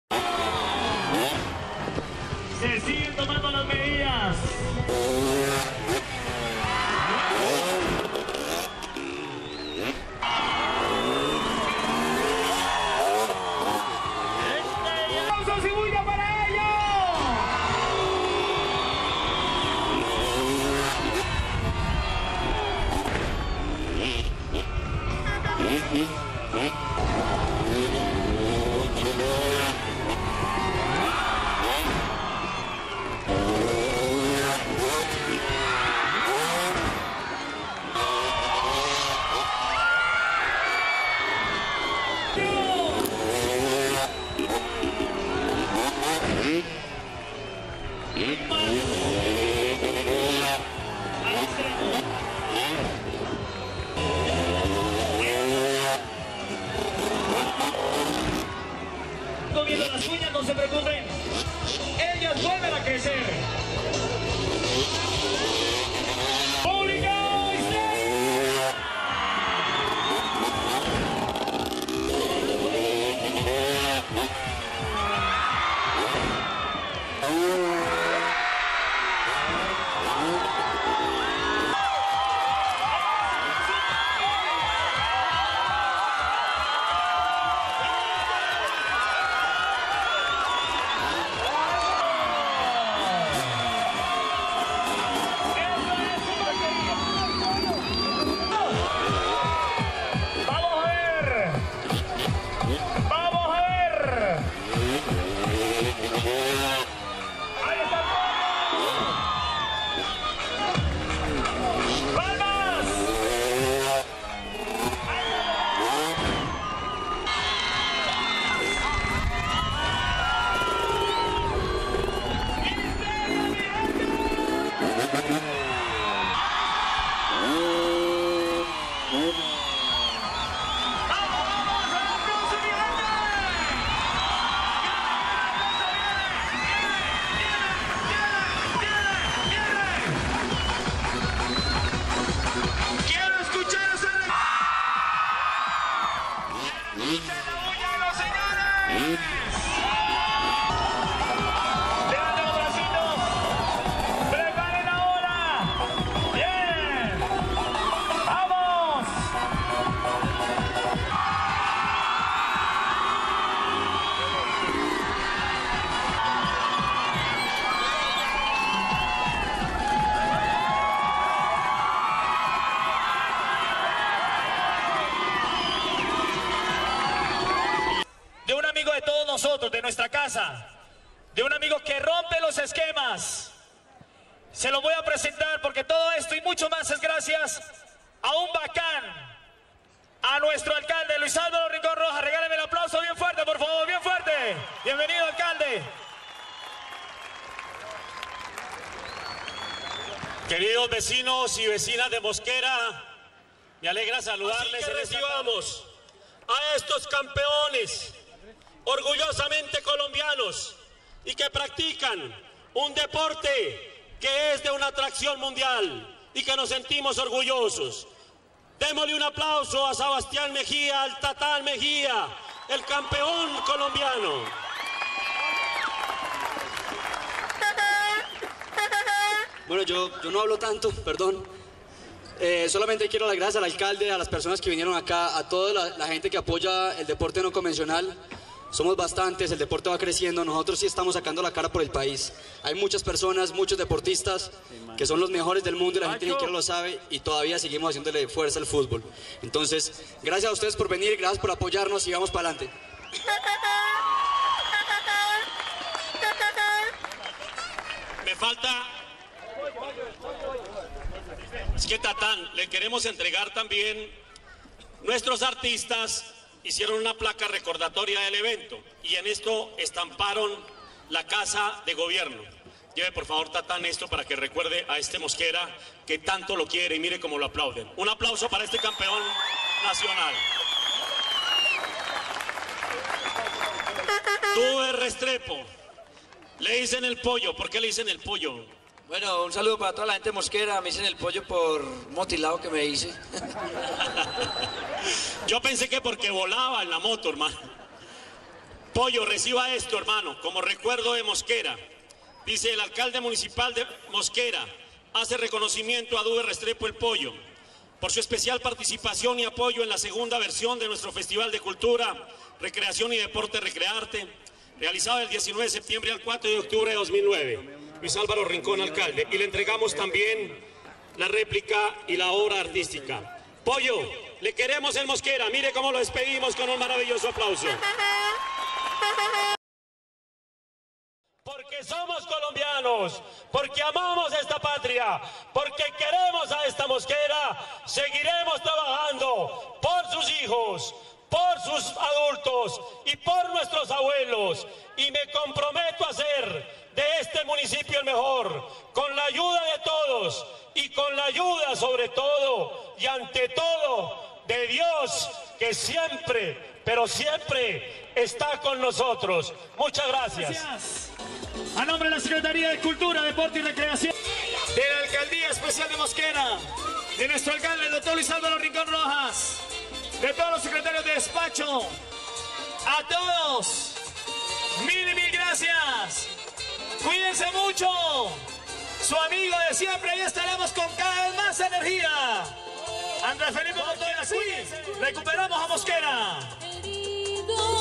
MOSQUERAtv TATAN MEJIA Y EL POLLO RESTREPO EN MOSQUERA CUND.MP4
MOSQUERAtv TATAN MEJIA Y EL POLLO RESTREPO EN MOSQUERA CUND
les presenta gracias al material suministrado por MOSQUERArv, el testimonio de la visita hecha al municipio de mosquera cund, por el Sr TATAN MEJIA Y EL POLLO RESTREPO, dos campeones de motociclismo en figuras acrobaticas orgullosamente colombianos.
ASI RECUPERAMOS A MOSQUERA